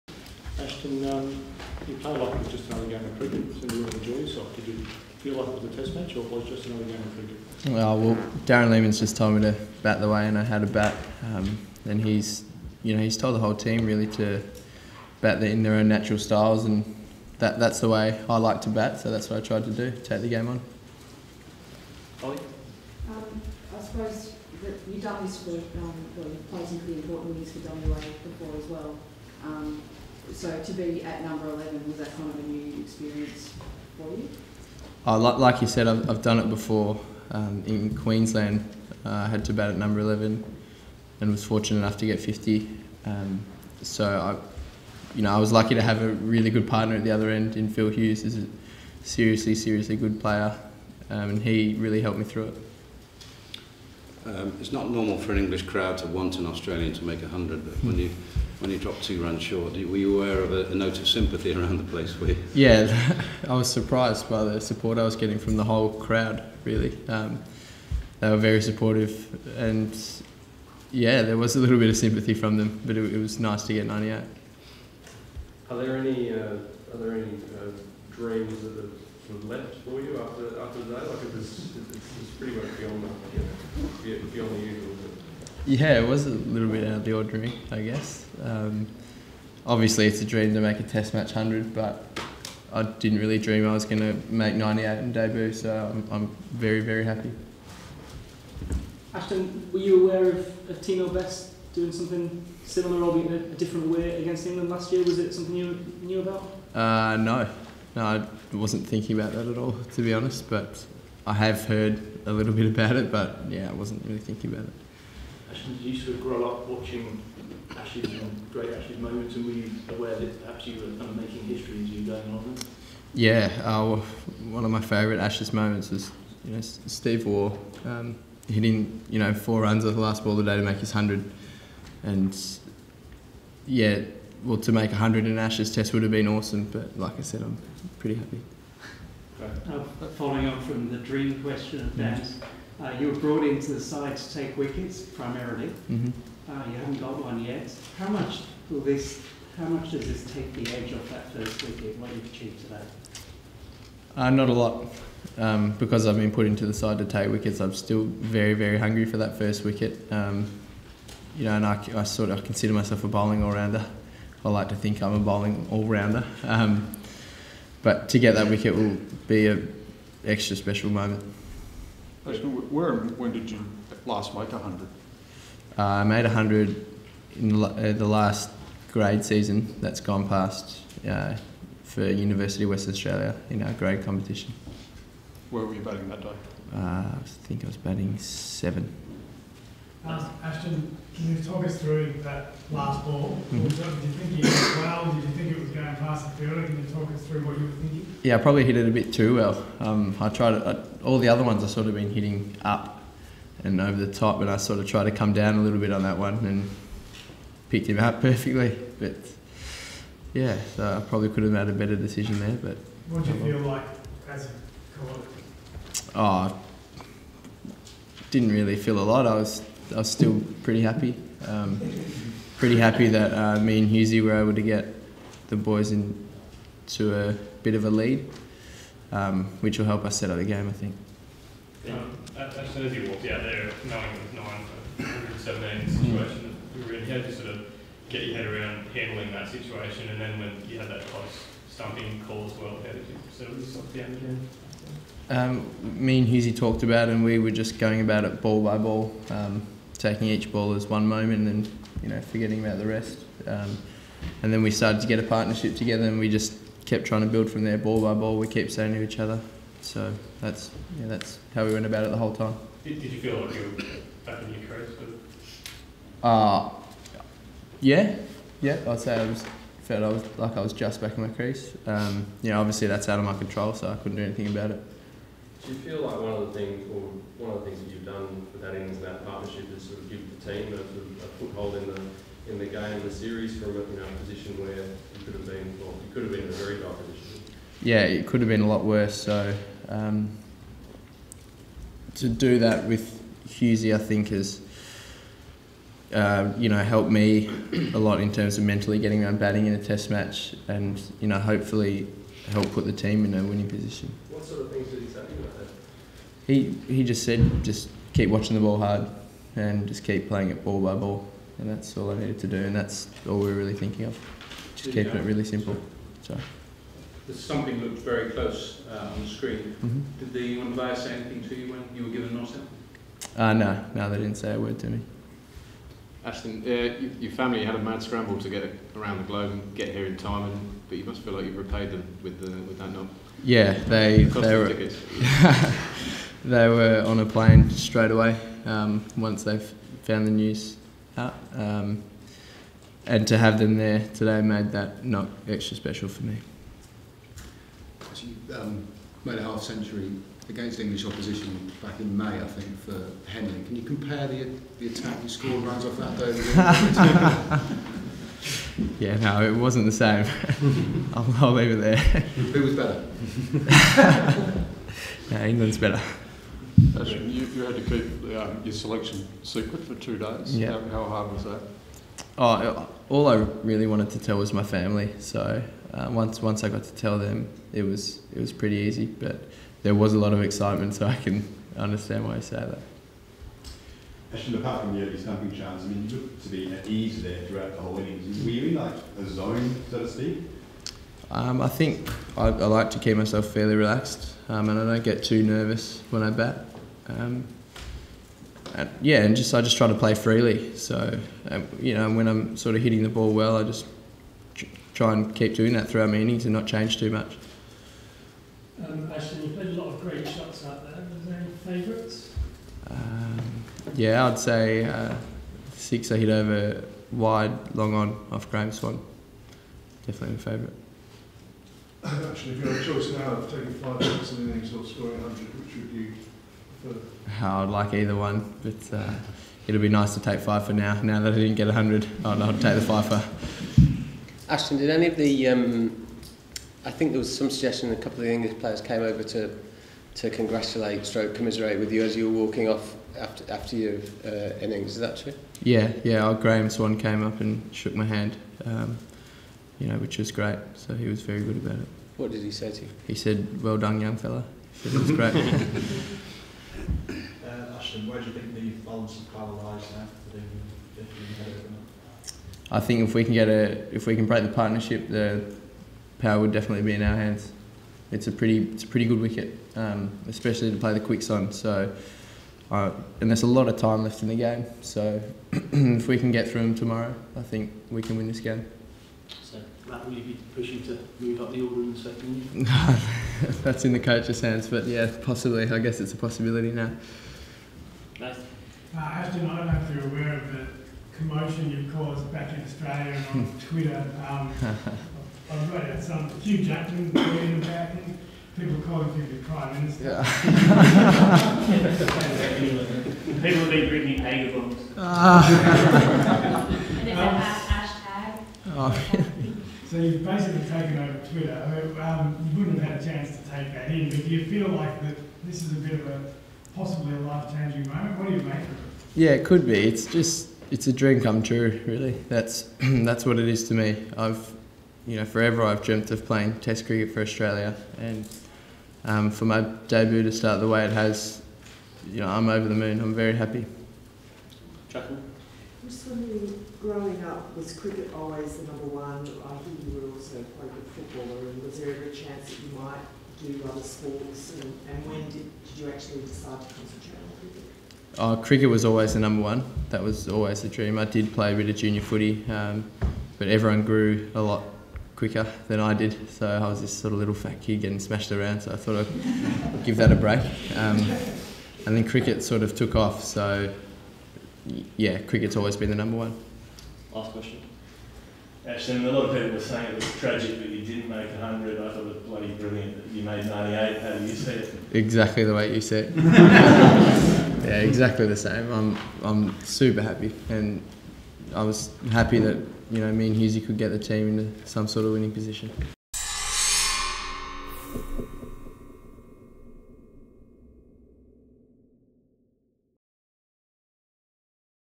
Life changing moment for Agar
Ashton Agar spoke to the media after his record-breaking innings on day two at Trent Bridge.